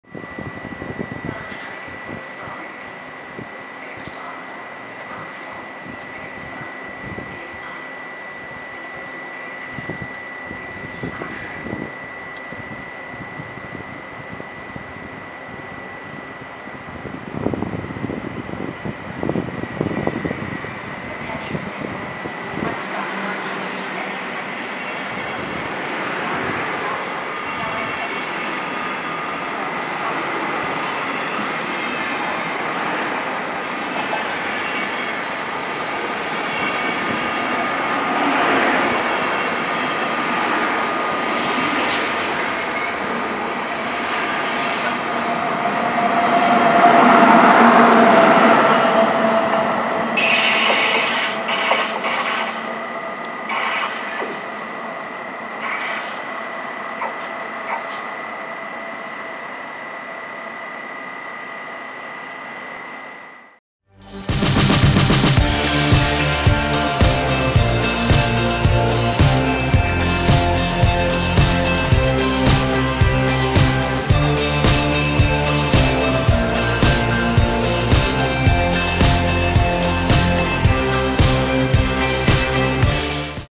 DB Bpmbdzf / BR101 - EC 114 WÖRTHERSEE - Salzburg Hbf
-(D): EuroCity 114 Wörthersee aus Klagenfurt Hbf nach Dortmund Hbf fährt ab von bahnhof Salzburg Hbf.
-(EN): EuroCity 114 Wörthersee from Klagenfurt Hbf to Dortmund Hbf leaving the station Salzburg Hbf.
-(SK): Medzištátny vlak EuroCity 114 Wörthersee (Klagenfurt Hbf ► Dortmund Hbf) odchádza zo železničnej stanice Salzburg Hbf. Súprava vlaku je tvorená vozidlami nemeckého národného dopravcu Deutsche Bahn (DB), pričom v čele vlaku je riadiaci vozeň rady Bpmbdzf a na konci súpravy elektrický rušeň 101.133.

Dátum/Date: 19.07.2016
Čas/Time: 12:09
Miesto/Location: Salzburg Hauptbahnhof (Rakúsko/Österreich/Austria)
Vozidlá/Vehicles: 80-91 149, 101 133,